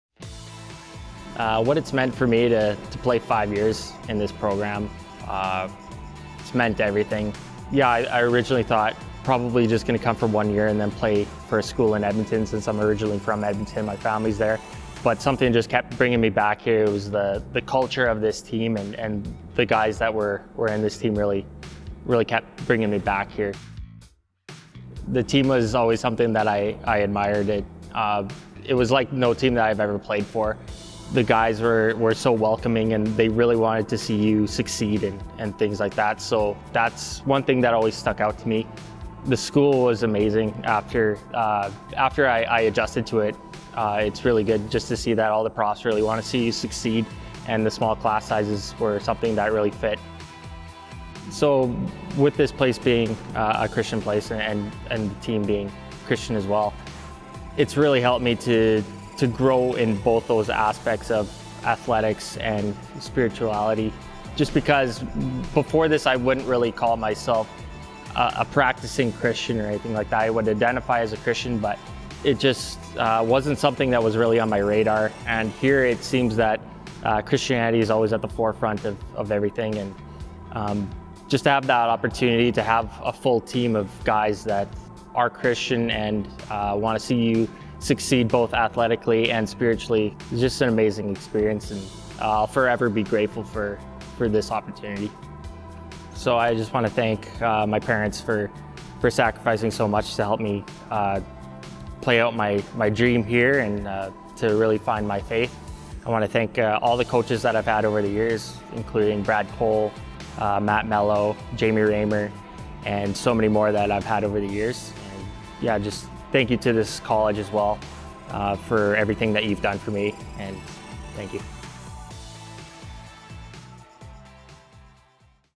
Chad Davidson | Briercrest Men's Hockey
Chad Davidson speaks to his experience of playing on the Briercrest Men's Hockey team for 5 years.